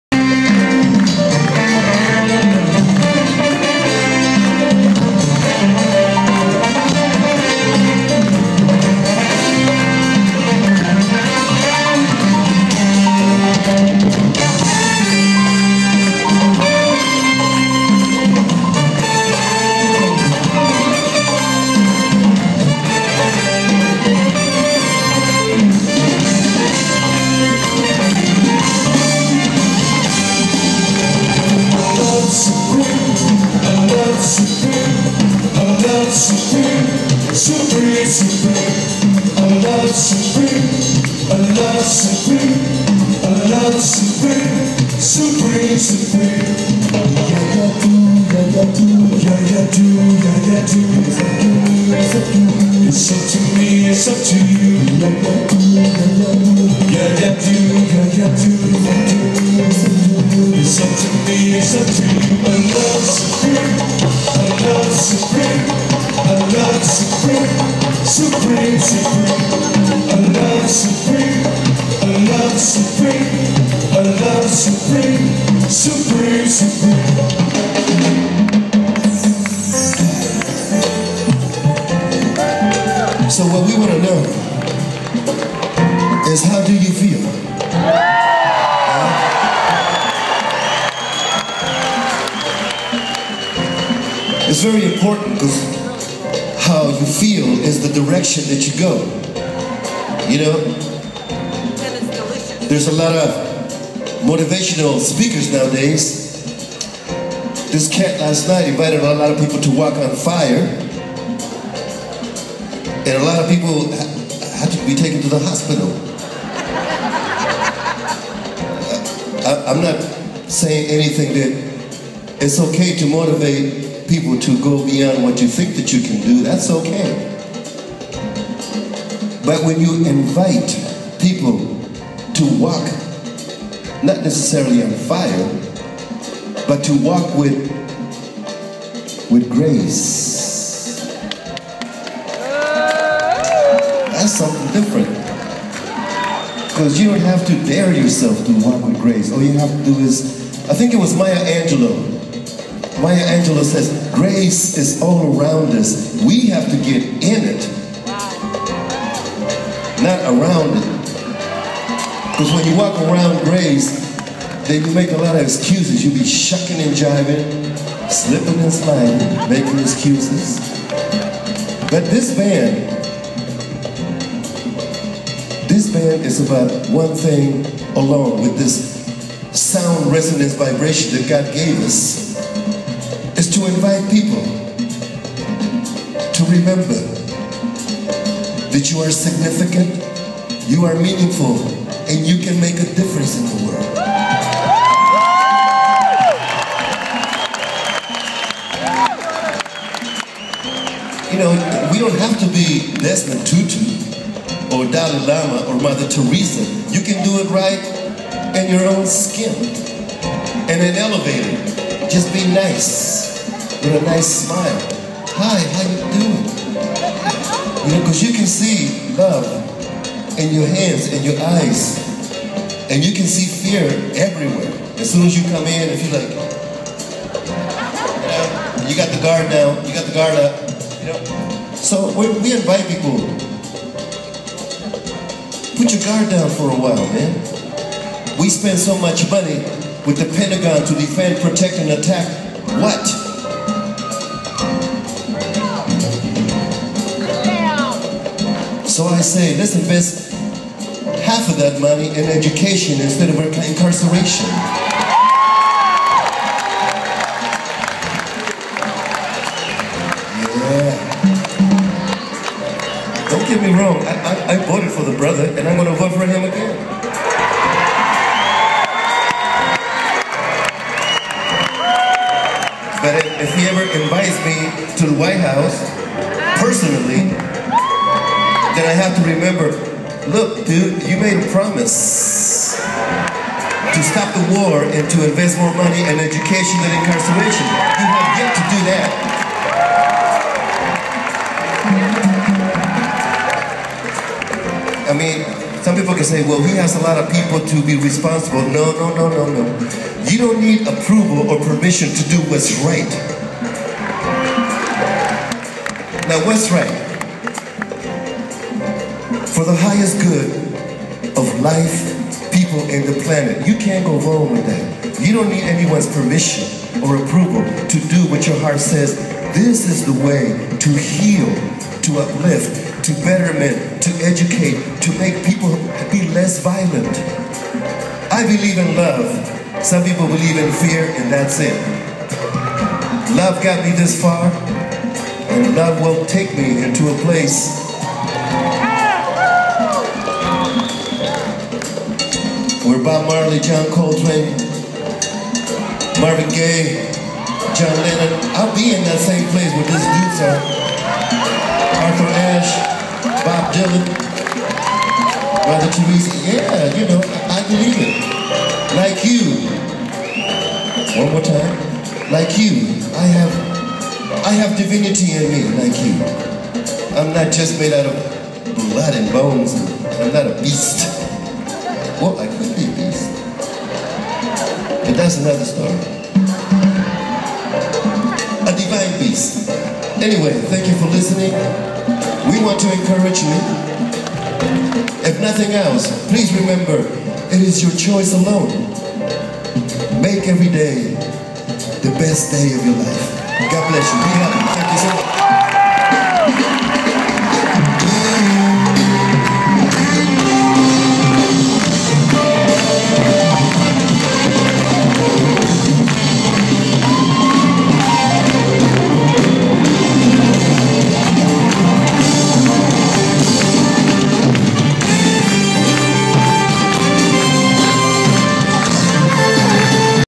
Carlos Santana July 21st 2012 in Atlantic City
Carlos Santana on the show in Atlantic City casino "Borgata" by July 21st 2012. I love his music and agree with his philosophy. I did recorded it because of his speech, not only for the show.